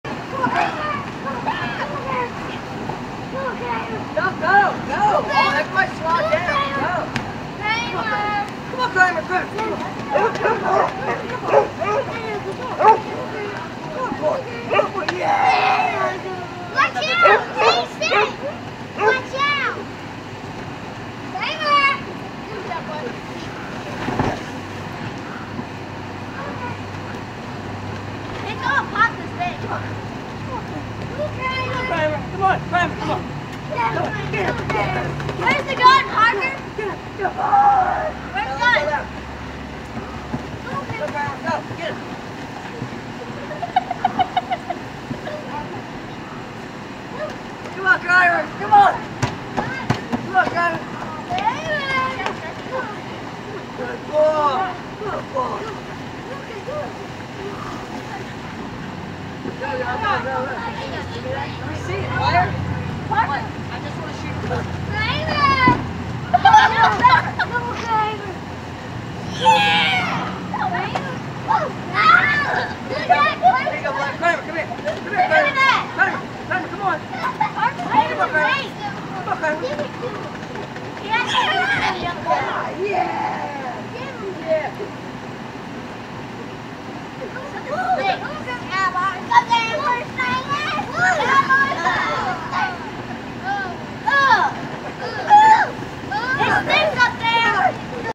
Pembroke Welsh Corgi on Slide with Kids
Our Pembroke Welsh Corgi "Kramer" on the air slide with the kids.